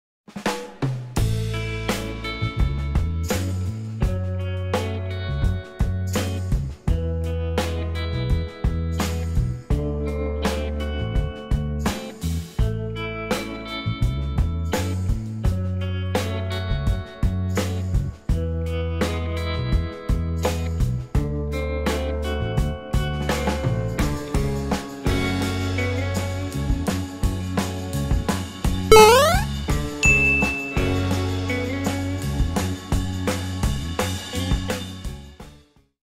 DIY Bird Trap Technology - Trapping Quail Bird With Creative Boys Trailer
This is the trailer for the second video on Green World channel about two children catch quail bird on the farmland. For more interesting video about bird trap, hunting, make bird trap, fishing or travel...